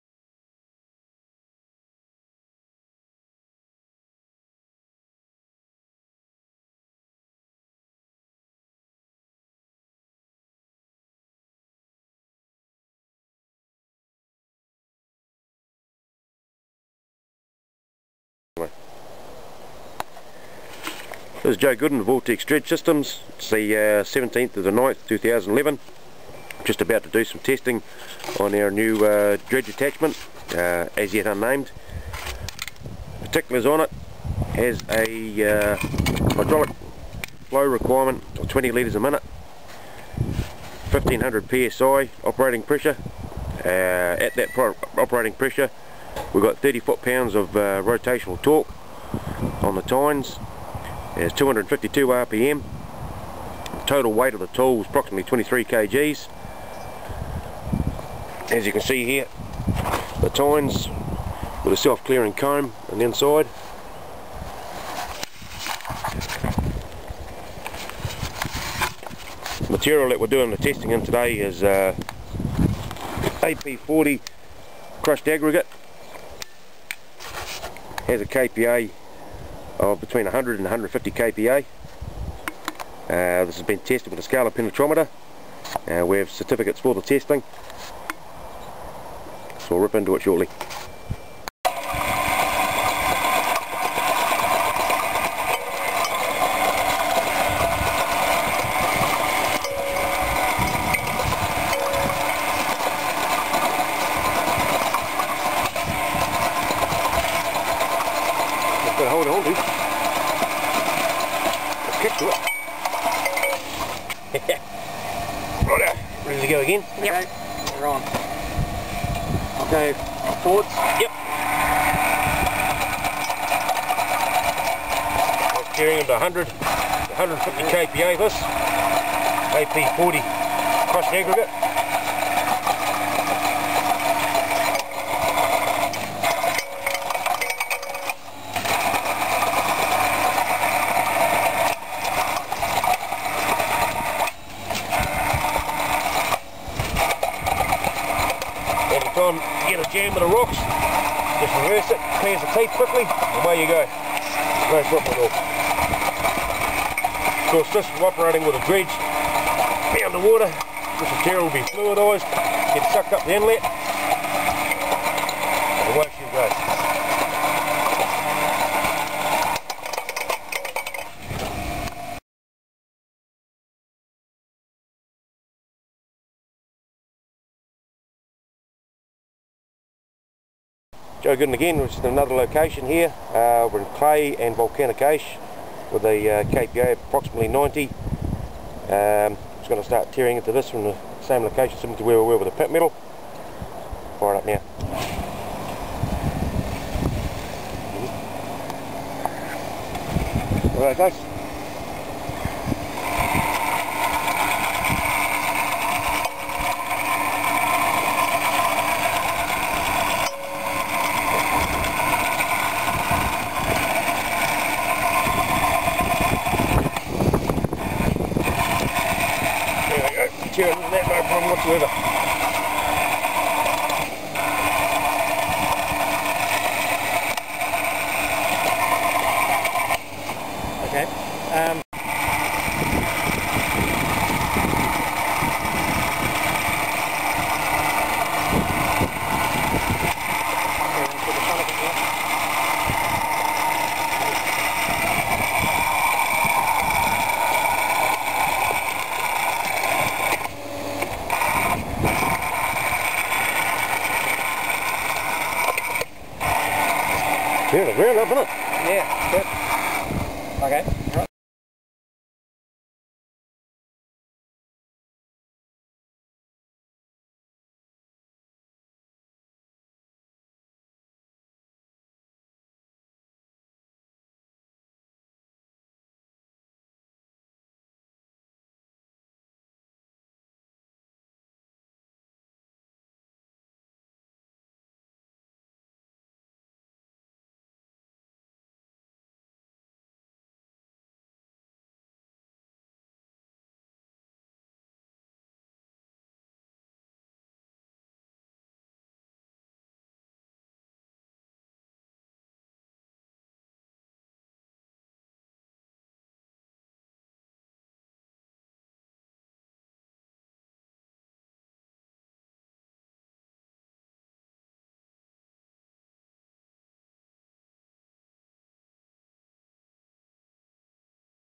Vortex Marine Rotary Hoe
This new tool from Vortex is designed to break up very difficult seabed soils and has just been through trials on land in soil tested and rated at 90 kPa and AP40 crushed aggregate rock / sand at 100 to 150kPa.
Fitted with 6 inch male cam lock with adapters to simply retrofit to the inlet hose of 3, 4 and 6 inch vortex dredges.
Hydraulic operation provides powerful 30 ft/lb rotary torque combined with 250 tyne rpm designed to fluidize soils with water for transition through dredge away from work area.
to view our product range.